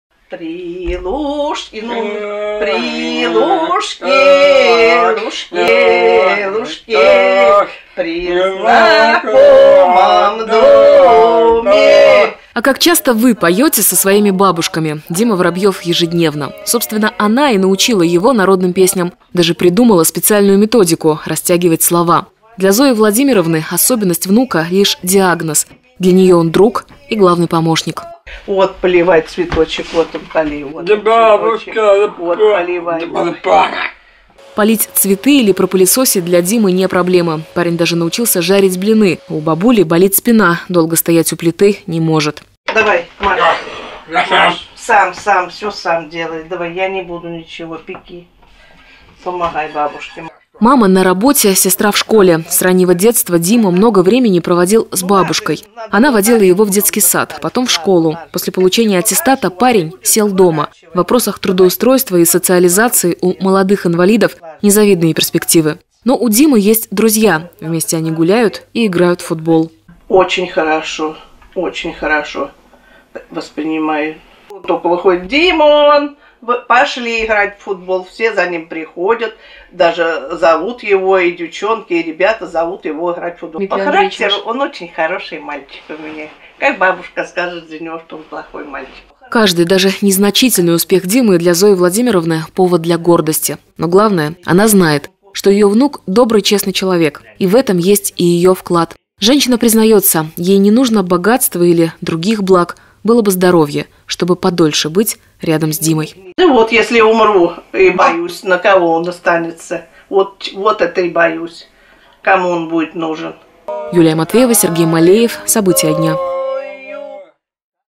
Курянка Зоя Воробьева воспитывает особенного внука
Жизнь мамы особенного ребёнка — не подарок, а ежедневный самоотверженный труд. Но некоторым везёт, на помощь приходят бабушки. Кто печёт самые вкусные пирожки и читает любимые сказки? Кто всегда поймёт и пожалеет? Курянин Дмитрий Воробьёв, у парня синдром Дауна, уверенно ответит — это его бабушка. В гости к особенной семье отправилась Юлия Матвеева.